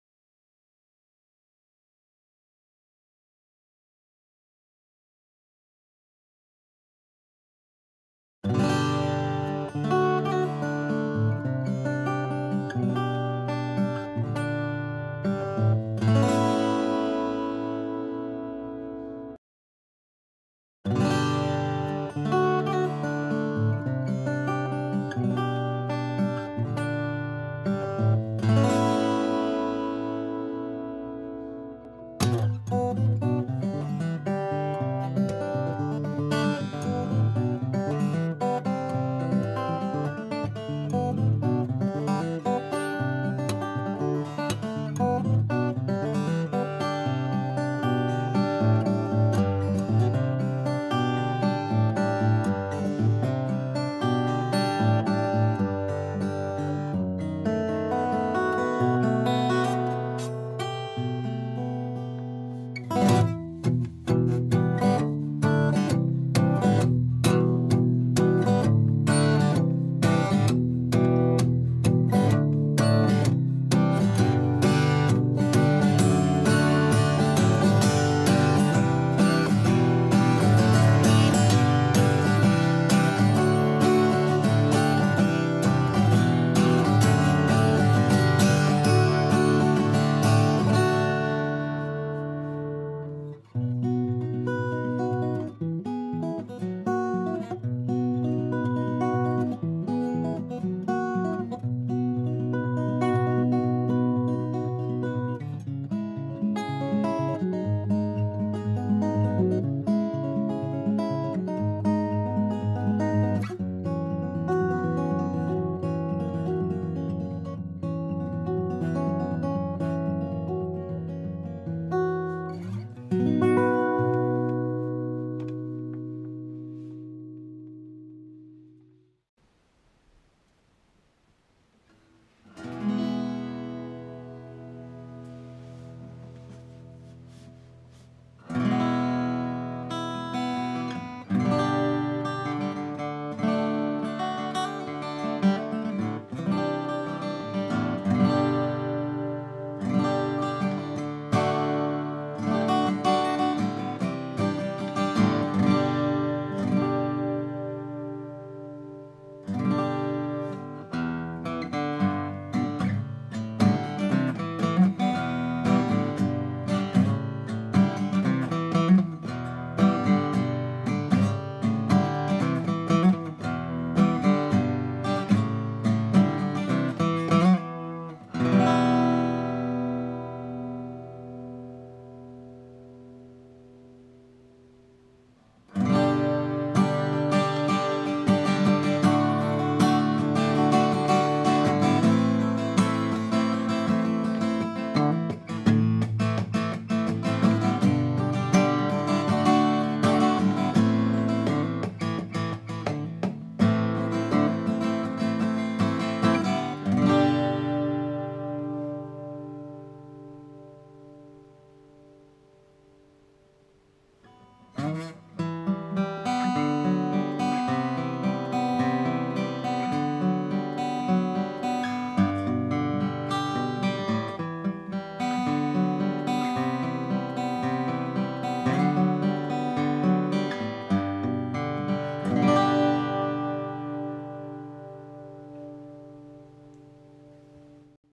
Furch OM 20 CM 2012
***SOLD***VENDUE***

All solid woods. Hand made in Czech Republic
Cedar top, mahogany back and sides
Mahogany neck and rosewood fingerboard

Comestic : 2 little dings on top and 3 little holes in the back of the neck. (see the video)